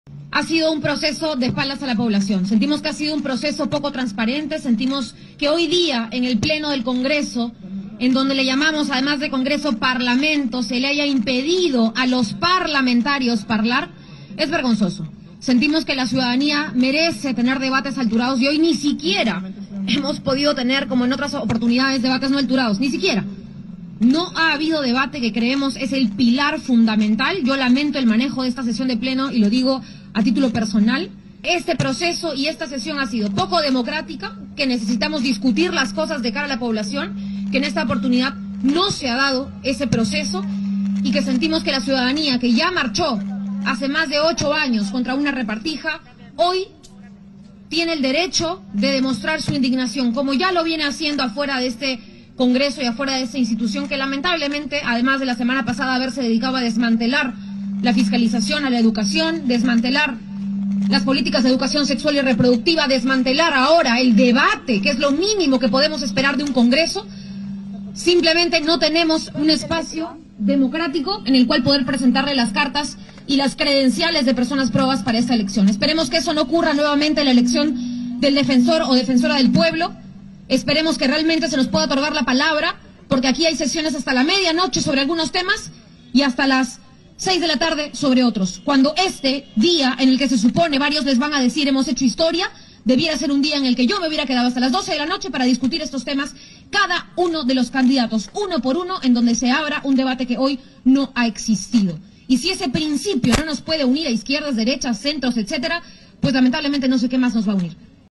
Sigrid Bazán cuestiona elección de miembros del TC: “Ha sido poco transparente”
Congresista Sigrid Bazán cuestiona elección de miembros del TC: “Ha sido poco transparente”
También criticó que el pleno del Congreso no debatió antes de la elección de los nuevos magistrados del Tribunal Constitucional.